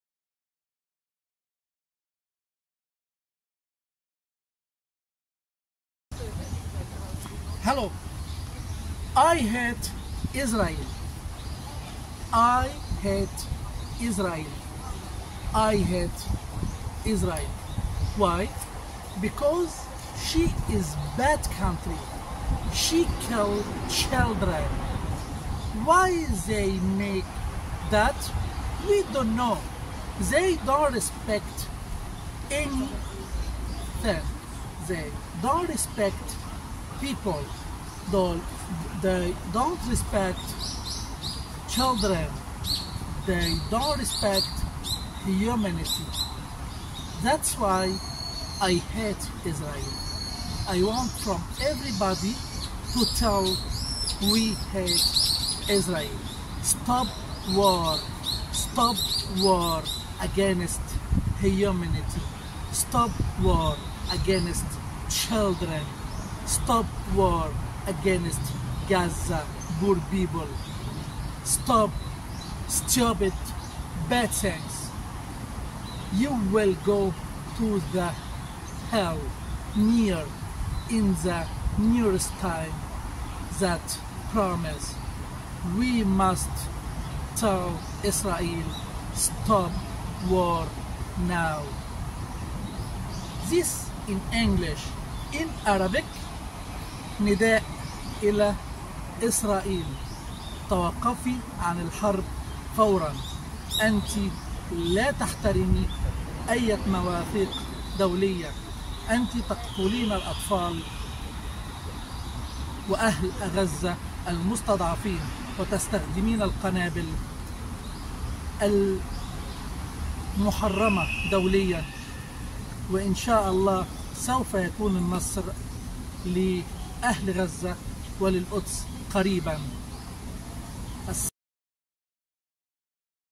رسالة لاسرائيل امام عدد من اليابانيين .. بالانجليزية من طوكيو اوقفوا الحرب علي غزه
انا بكره اسراeل رسالتي  بالانجليزية من طوكيو امام حشد من اليابانيين